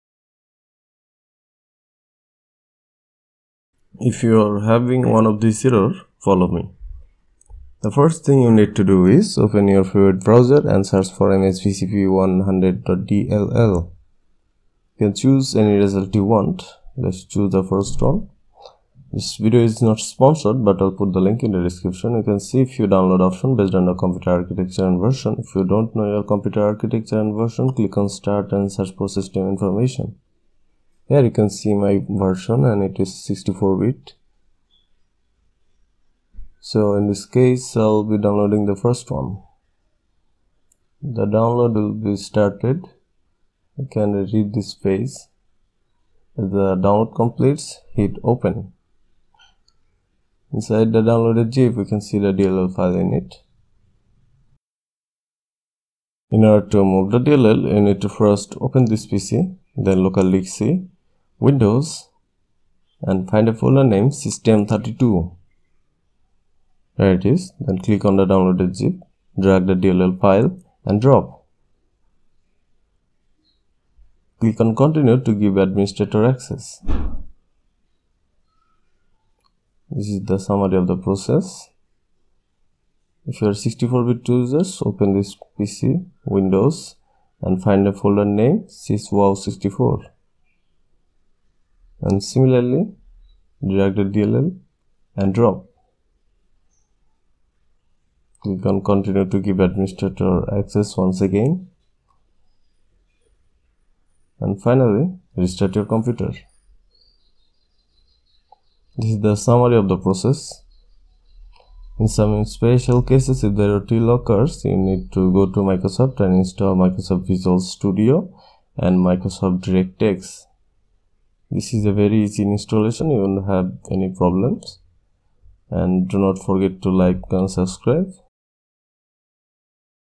2023 How to Fix MSVCP100.dll Missing from computer / Not Found 🎮GTA V Error 💻 Windows 11/10 32/64bit
The program can't start/ code execution can't proceed msvcp100.dll error for PES, Call Of Duty. Far Cry, Sims4, Assassin's creed, Borderlands etc. are fixed with this method.

So, you can't open your previously installed game or high graphics program. This error message "the program can't start because msvcp100 is missing from your computer" arrive due to failure in microsoft visual c++ redistributable. So longer practice is to install or update the latest visual c++, available in microsoft's official website. But we can also place the missing msvcp100.dll file.

So basically
first you need to know your windows version and architecture
then search msvcp100.dll  or click on the link I provided above
Next download your version
after that place msvcp100.dll file to C:\windows\system32 folder and also to C:\windows\sysWOW64 (only 64 bit user)
finally restart
By doing all these, your msvcp missing problem will be fixed.

TimeStraps
0:00 Intro
0:11 The issue
0:21 Alternate fix
0:31 Download msvcp100.dll
1:40 Fix msvcp100.dll 32 Bit & 64 Bit
2:12 Fix msvcp100.dll 64 Bit
2:53 Outro